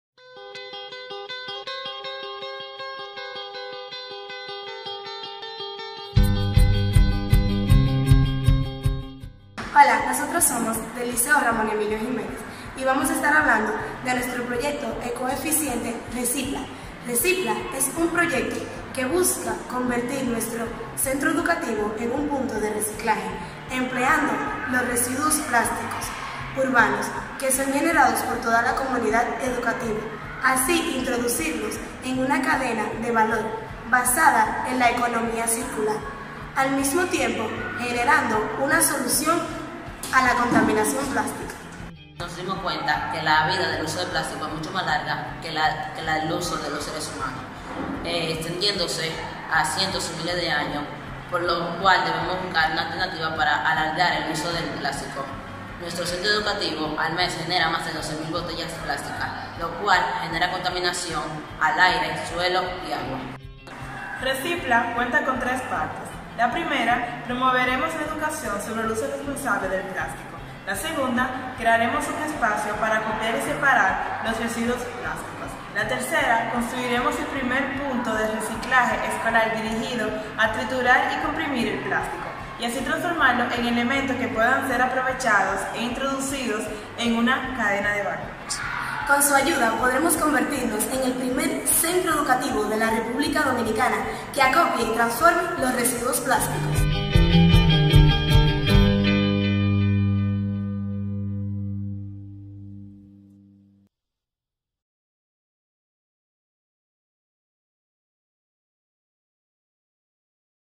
Liceo Ramón Emilio Jiménez - ¡Soy Soy Ecoeficiente!
Centro Educativo: Liceo Ramón Emilio Jiménez
Eje temático: Manejo de Residuos Sólidos y Cultura 3R
Proyecto: ReciPla
Colaboradores: Heidy de la Rosa Oliver, Kevin Jaime, Dominic Martínez, Genesis Reyes Santana.